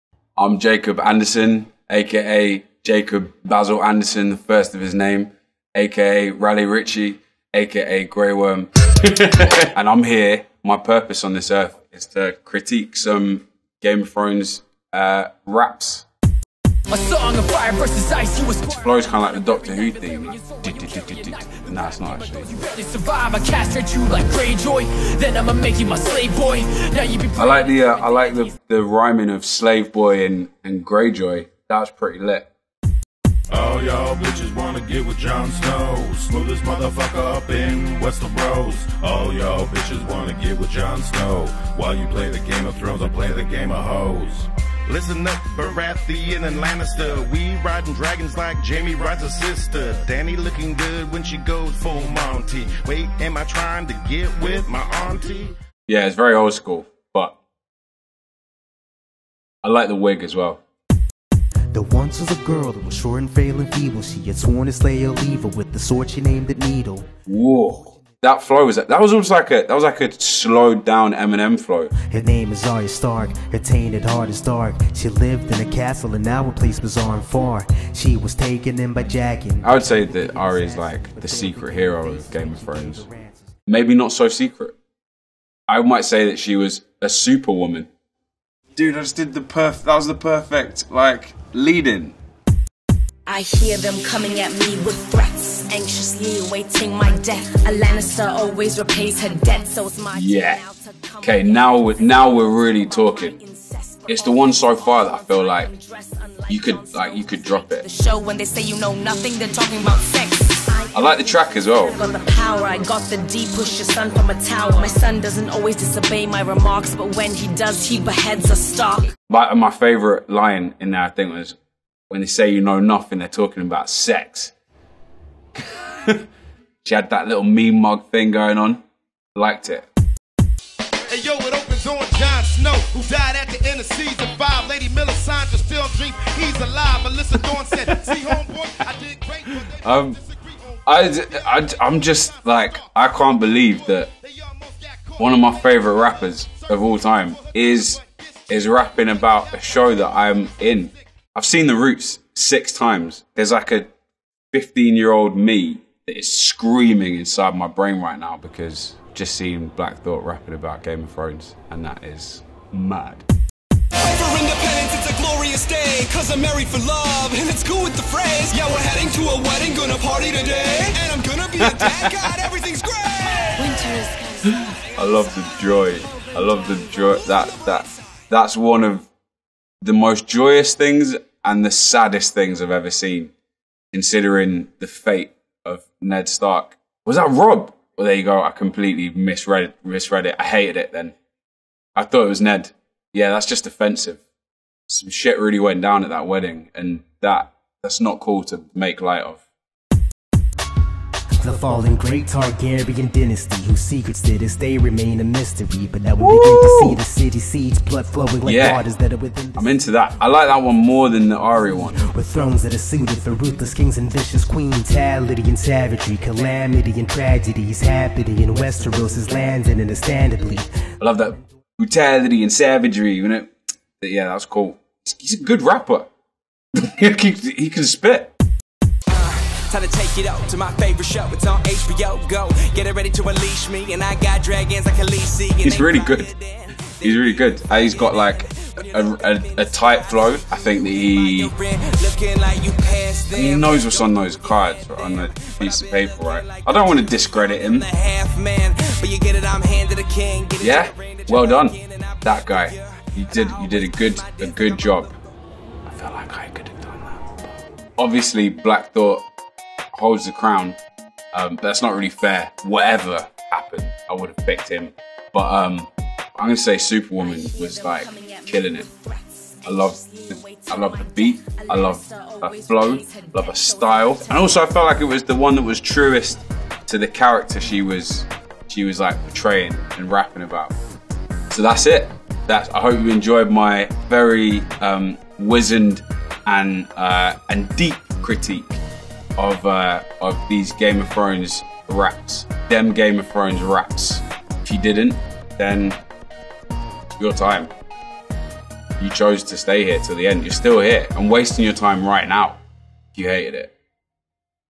Grey Worm Breaks Down Game of Thrones Raps | Vanity Fair
Jacob Anderson, who plays Grey Worm on Game of Thrones, reacts to raps about the hit HBO show.